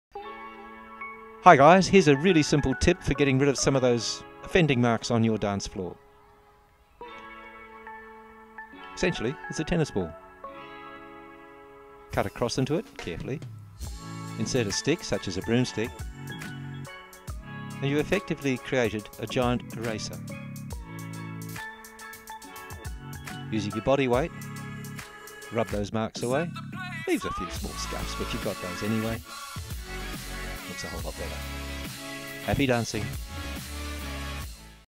STM Hack - How to get rid of scuff marks on your dance vinyl!
Remove scuffs from vinyl flooring with the housekeeping tip in this free video on home maintenance from a professional studio decorator and home cleaner.

In this video, we'll show you a simple and effective method for removing scuff marks from your dance vinyl using just a tennis ball. Whether you're a dancer studio owner, keeping your vinyl clean and free from scratches and scuff marks is essential to maintaining the quality of your practice and your performance.

To start, you'll need a clean tennis ball and your scuffed dance vinyl. Simply take the tennis ball and rub it gently over the scuff marks on the vinyl, using circular motions. Apply a bit of pressure to the ball to help lift the scuff marks, but be careful not to press too hard or you could damage the vinyl.

Continue rubbing the ball over the scuff marks until they start to fade away. Depending on the severity of the scuffs, you may need to repeat the process several times to get the best results.

Once you've removed the scuff marks, wipe down the vinyl with a soft cloth to remove any leftover debris or dust. Your vinyl should now be looking as good as new!

Overall, this method is a quick and easy way to keep your dance vinyl in top condition, and it's a great alternative to using harsh chemicals or expensive cleaning solutions. So grab a tennis ball and give it a try!
The STM Hack.